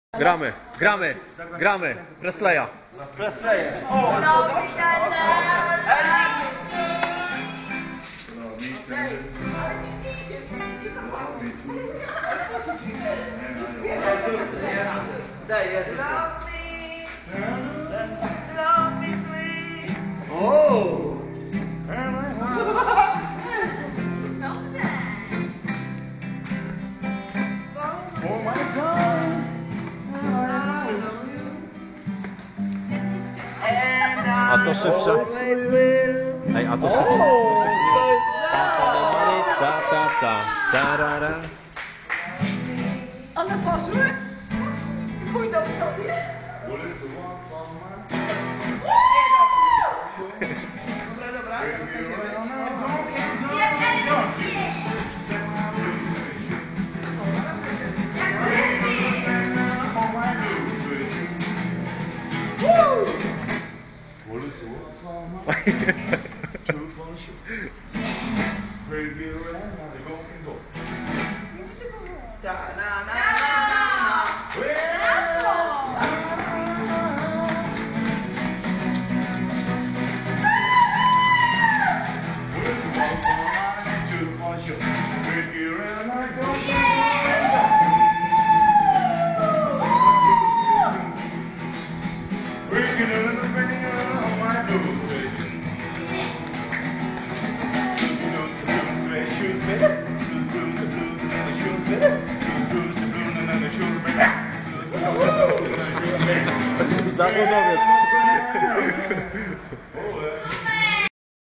Przejscie Swidnickie Duch Dwooch Presleyidziewki
Kolejny poranek po nocy grania na Przejsciu Swidnickim, i pamietajcie rozne dziwne rzeczy sie tam dzieja takze wchodzicie na wlasne ryzkyko hahahahahahaha ;-